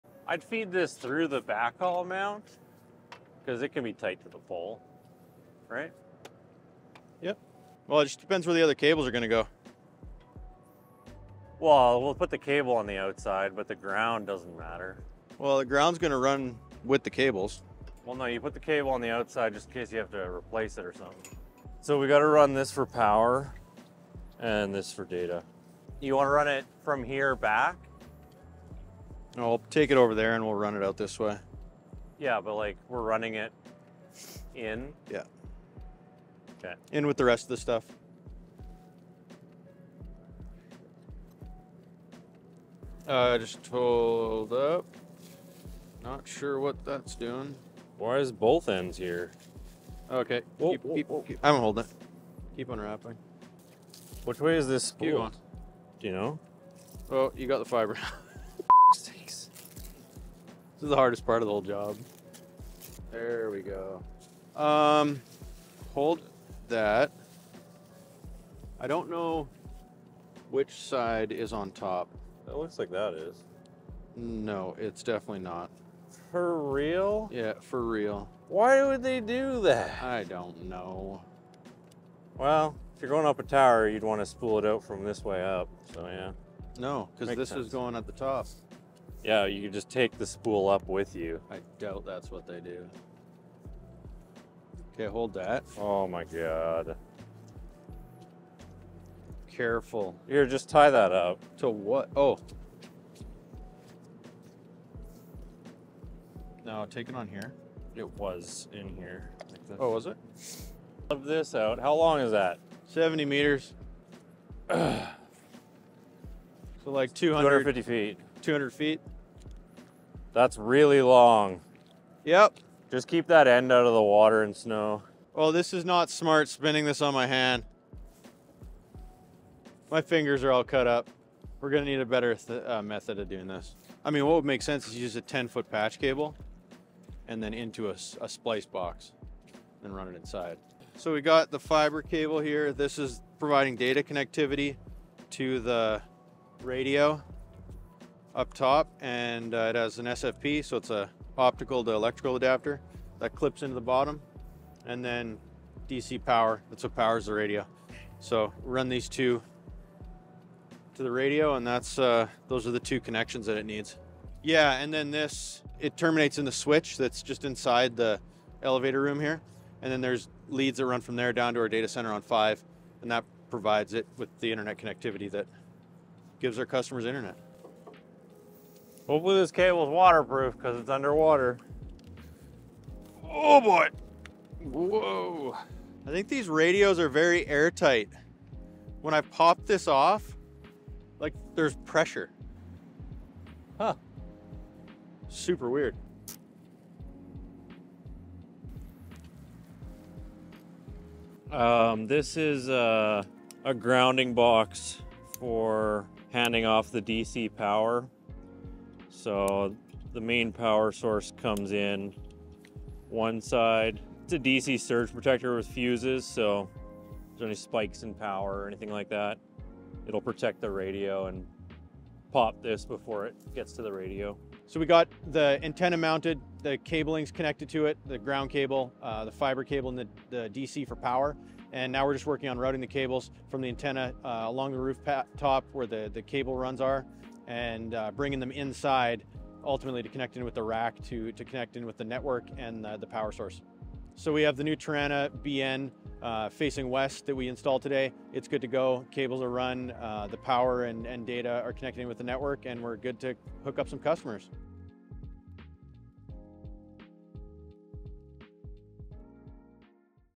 Tarana Wireless Deployment - Regina, SK - Part 3
Follow us as we deploy a new high-speed wireless internet network in Regina, SK, Canada. We are bringing residents more cost-effective options for their home internet service by delivering it via next generation wireless-to-the-home technology powered by Tarana wireless hardware.